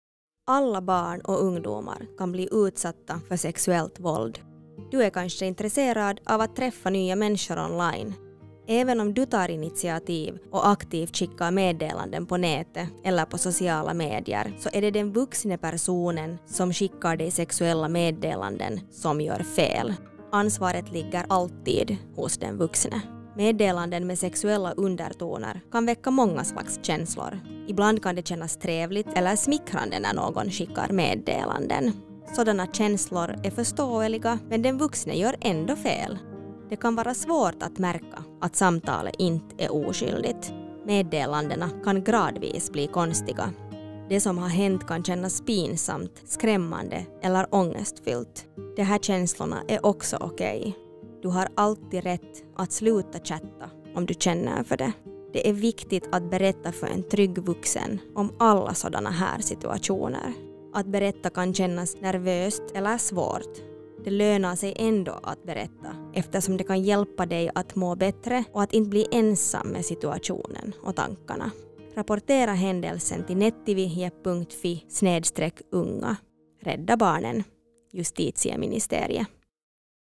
Sexuellt våld kan vara svårt att känna igen
Berätta alltid för en pålitlig vuxen om du stöter på något misstänkt på nätet.